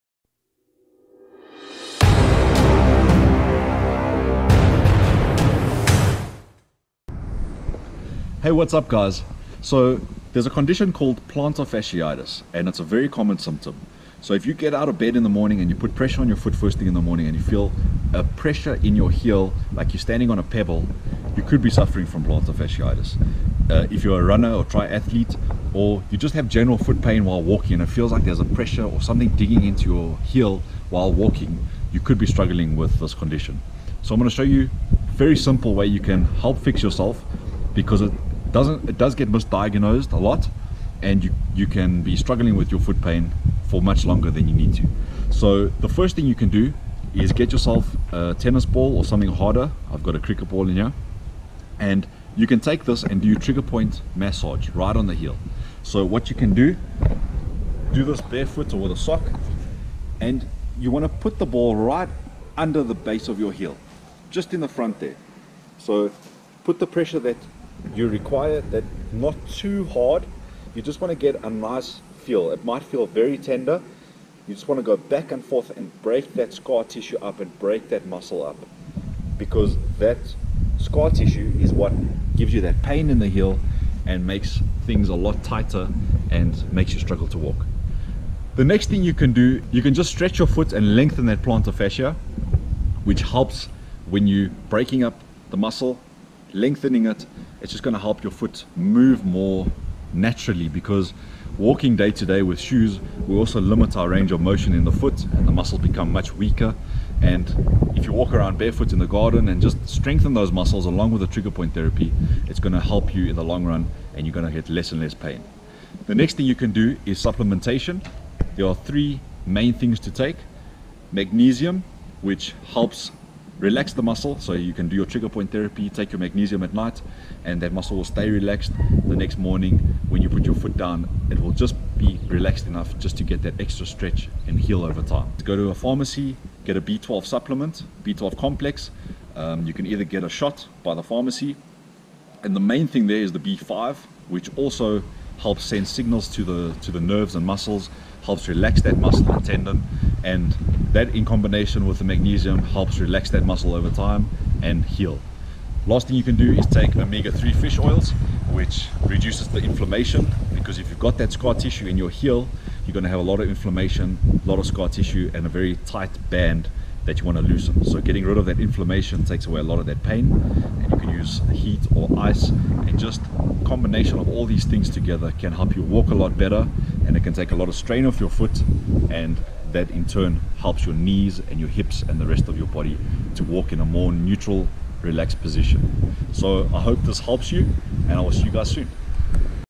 FIX YOUR PLANTER FASCIITIS NATURALLY AND EASILY
Plantar fasciitis is one of the most common causes of heel pain. It involves inflammation of a thick band of tissue that runs across the bottom of your foot and connects your heel bone to your toes (plantar fascia).

Commonly causes stabbing pain that usually occurs with your first steps in the morning. As you get up and move more, the pain normally decreases, but it might return after long periods of standing or after rising from sitting.

 It's more common in runners. In addition, people who are overweight and those who wear shoes with inadequate support have an increased risk of plantar fasciitis.